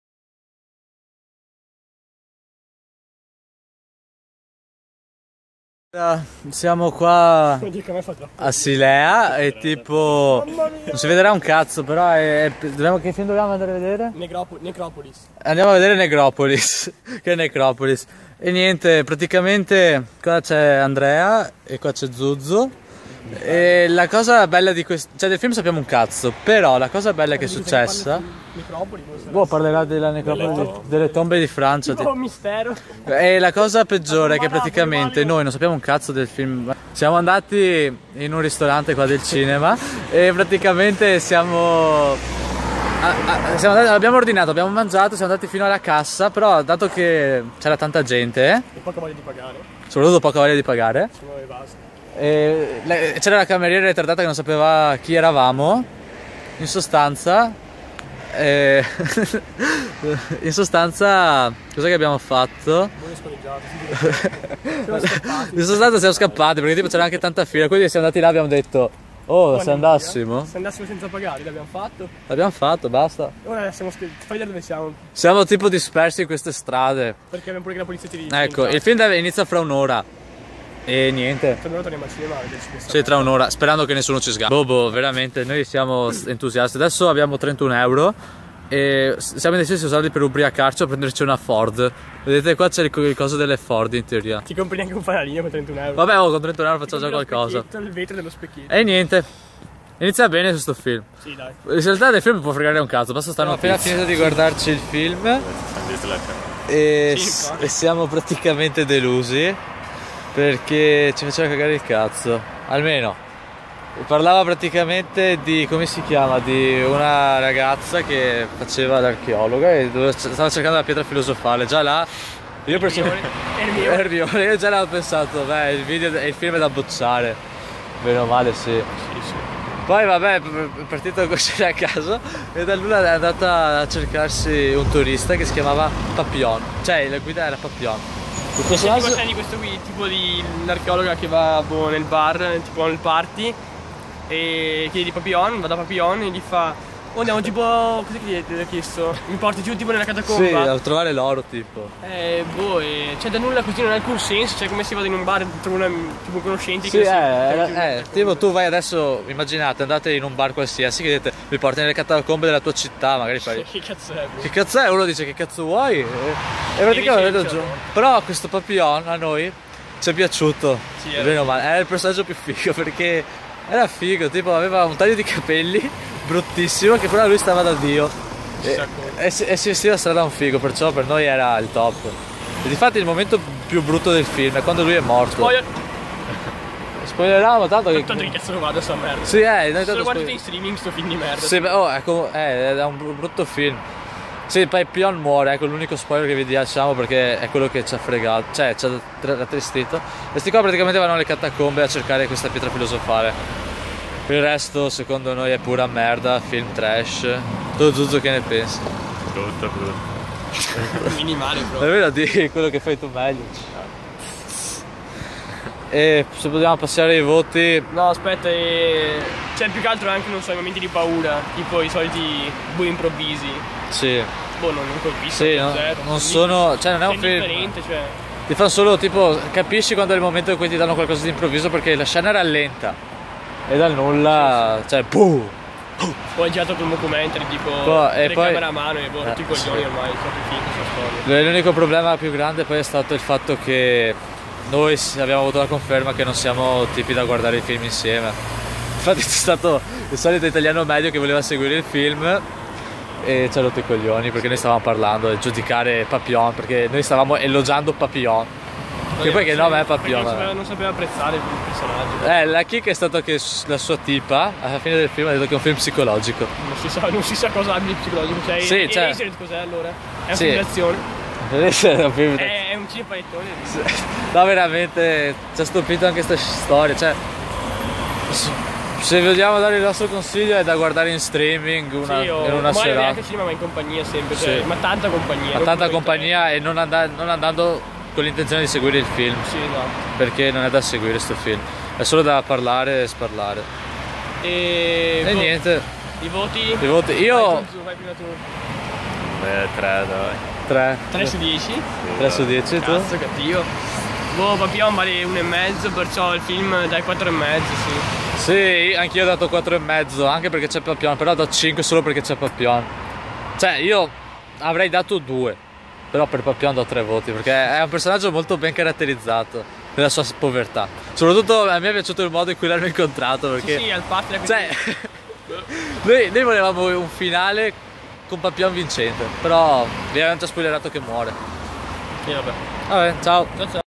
Recensione-Necropolis la città dei morti
Siamo andati a vedere il film a Silea, il film ci ha delusi assai pensavamo meglio. Il viaggio fino al cinema però è stato riconpensato grazie alla cena a parametro zero sennò sarebbe stata una serata da buttare via